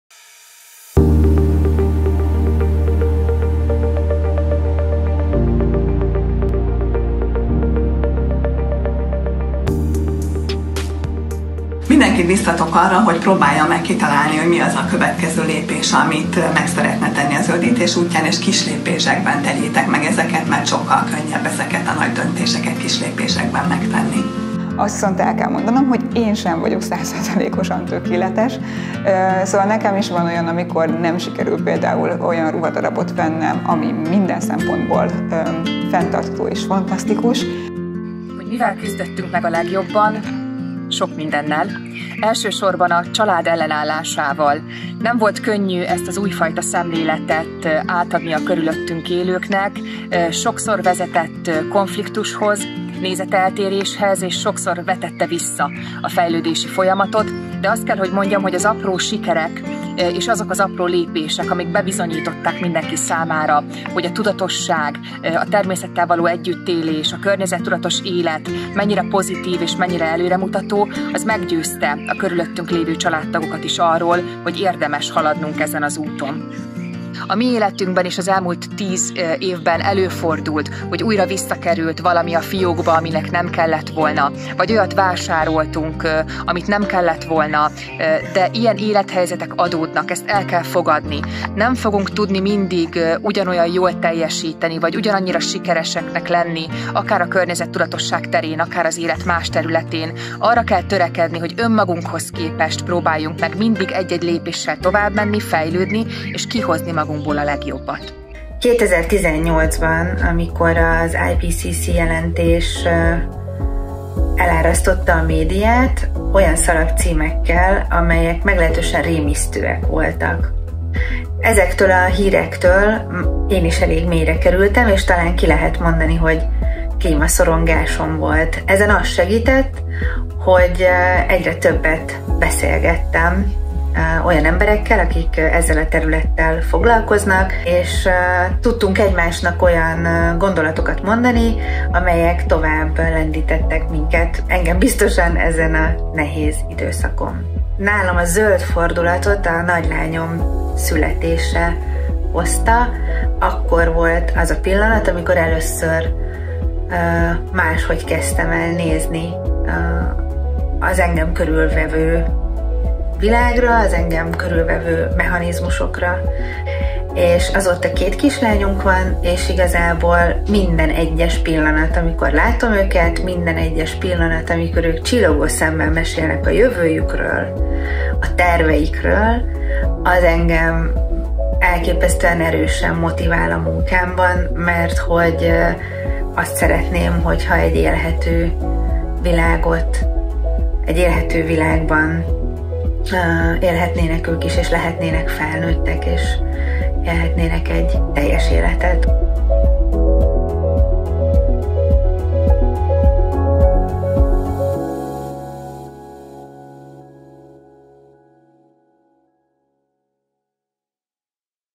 Az én környezettudatos utam #3 - Közösen könnyebb
Az én környezettudatos utam videósorozatban a Tudatos Vásárlók Egyesületéhez közel álló emberek mesélnek arról, hogy az életük különböző területein ők hogyan tesznek a fenntarthatóságért.

A Közösen könnyebb epizódban Ekler Éva, Mengyán Eszter (Holy Duck blog, fentarthatodivat.hu alapítója), Amrein Tamásné Miskolczi Boglárka és Nagy Réka (okoanyumag.hu alapítója)